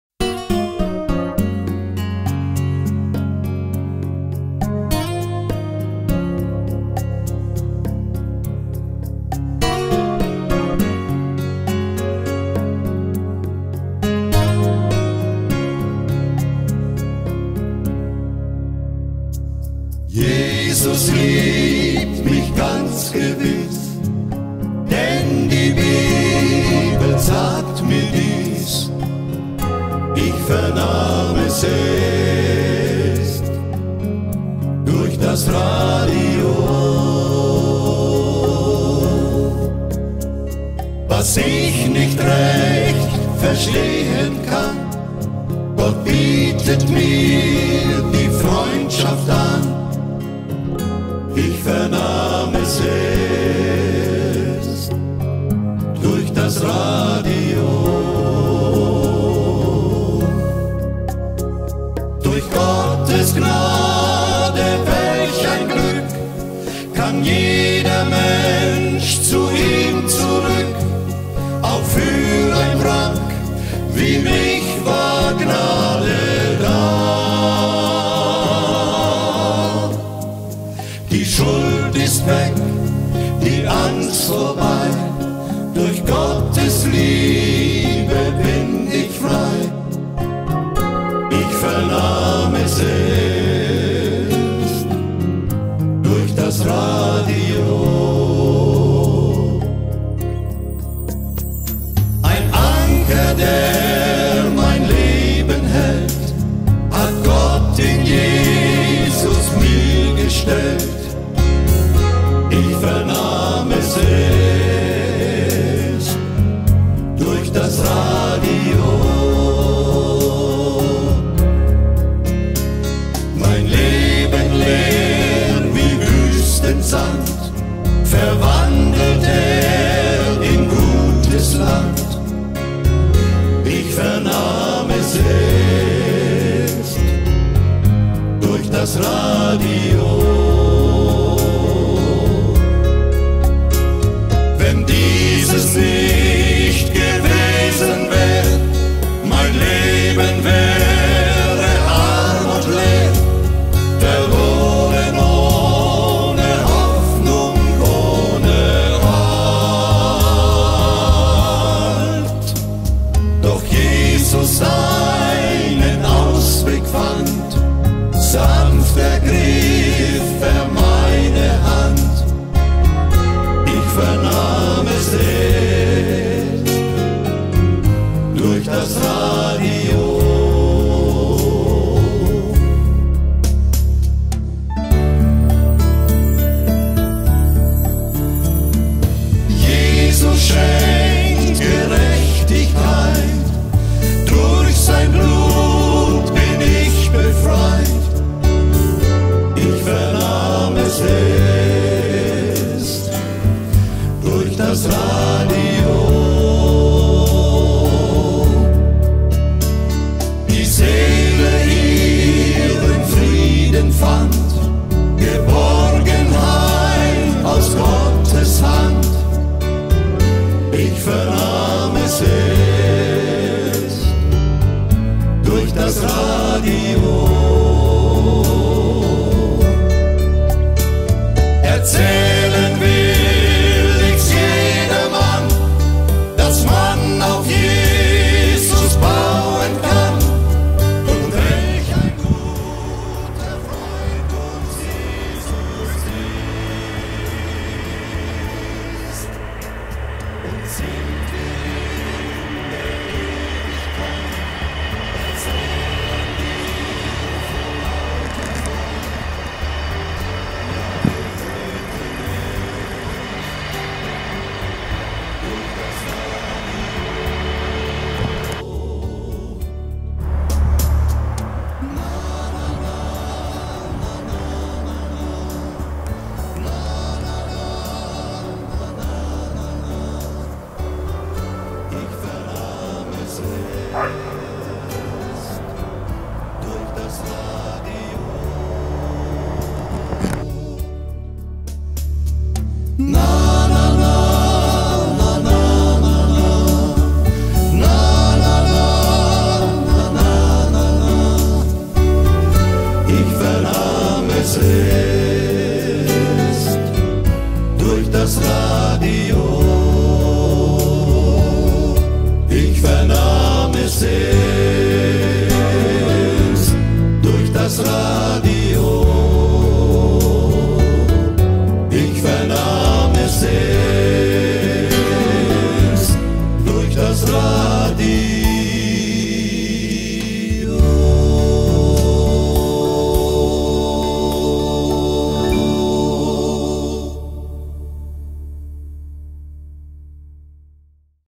"Ich vernahm es erst "m4v
Es wird Zeit, dass denkende und wahrheitsliebende Menschen auf die rettende und froh und glücklich machende Botschaft des Wortes Gottes hören. Sonst werden sie bald betrogen werden von dem Widersacher Gottes und "Lügner von Anfang an". Satan ist ein Betrüger und der bald erscheinende Antichrist, der Widersacher Gottes muss nicht nur den Abfall des Christentums sondern auch die Entrückung der wiedergeborenen Christen abwarten, bevor er, der "Gesetzlose" freie Bahn haben wird, die Menschen ins Verderben zu stürzen. ZU den Juden sagte Jesus: "Ich wollte euch sammeln, wie eine Henne ihre Küchlein sammelt, aber ihr habt nicht gewollt!" So müssen die nichtmessianischen Juden leider noch viel Leid erfahren, bevor ihr Messias am Ende des Äons Seine Füße auf den Ölberg stellen wird und sich ihnen zu erkennen gibt. (Sacharia 14, 4)..